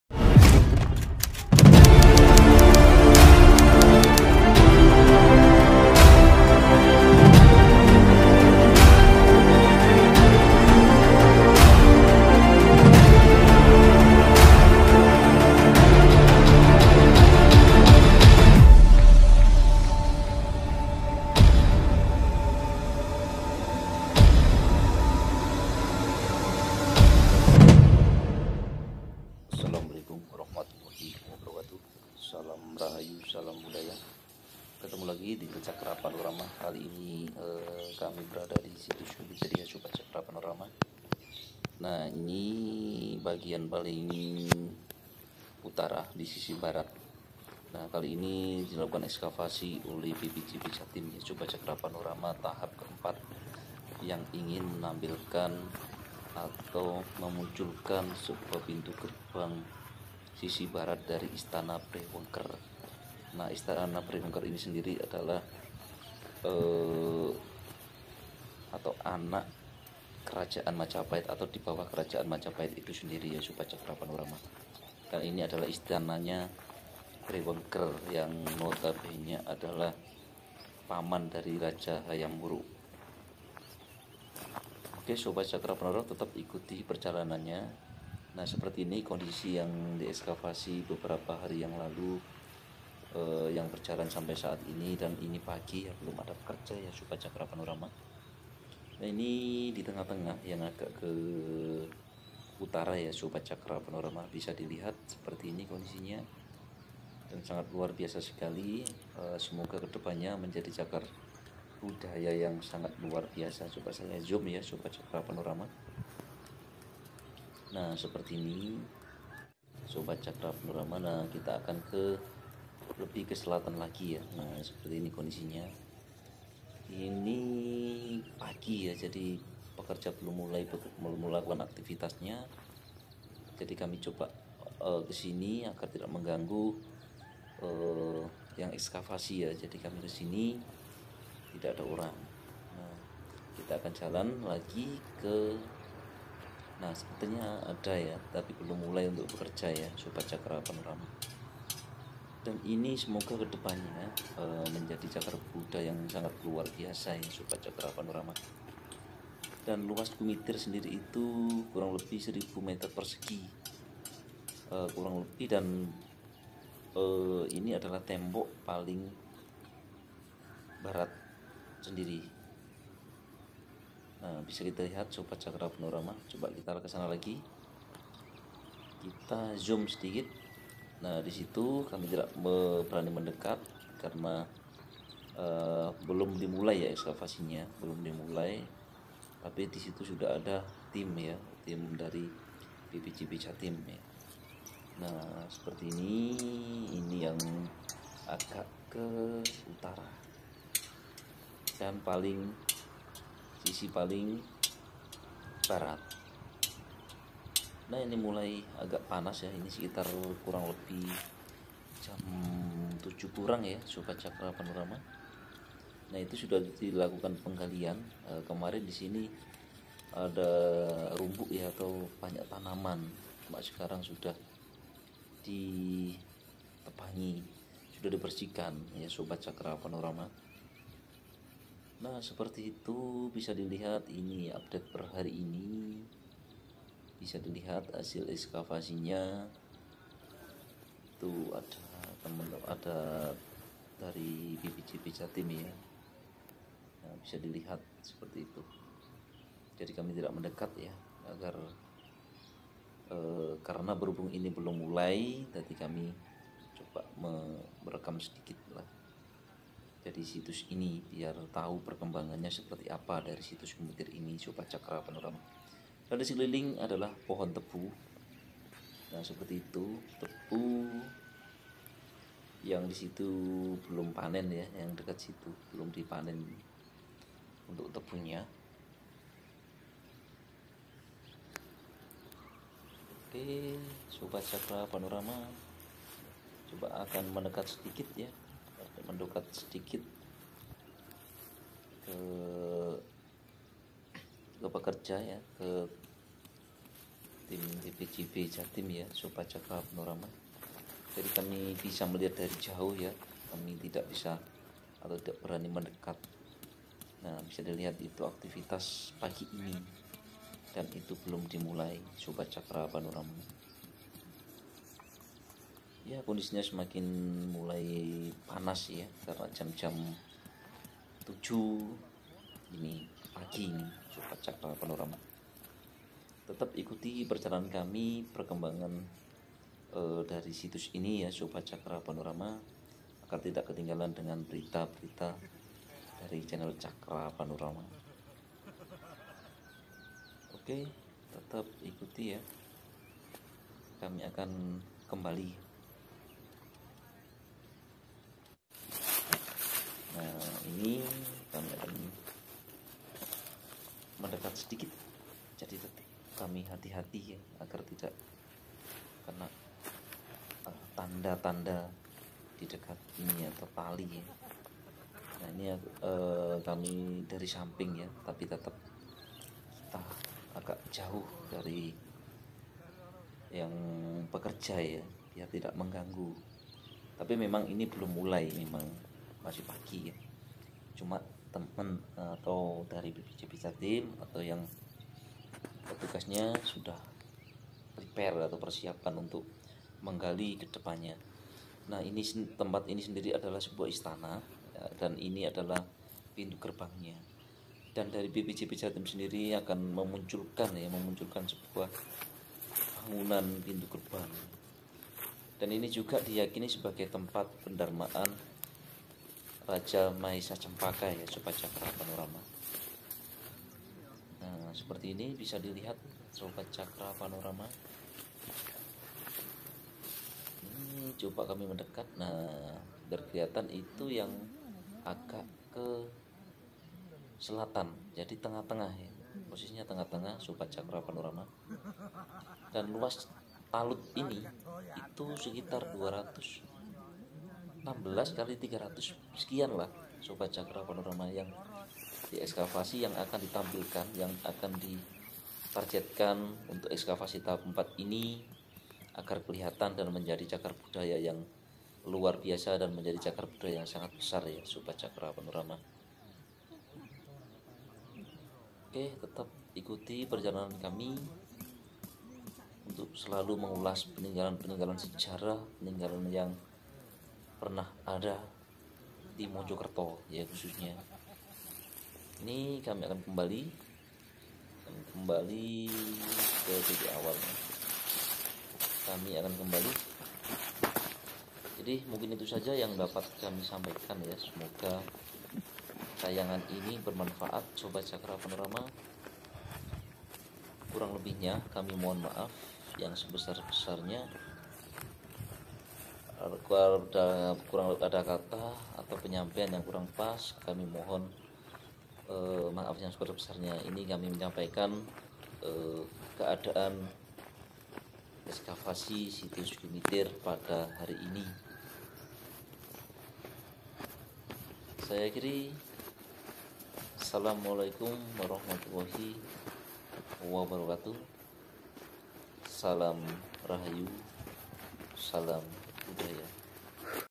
Ekskavasi Situs Kumitir, Setelah Beberapa Hari Digali Akhirnya Gerbang Istana MAJAPAHIT Mulai Nampak
Ekskavasi Situs Kumitir, Setelah Beberapa Hari Digali Akhirnya Gerbang Istana MAJAPAHIT Mulai Nampak - Jatirejo Mojokerto Jawa Timur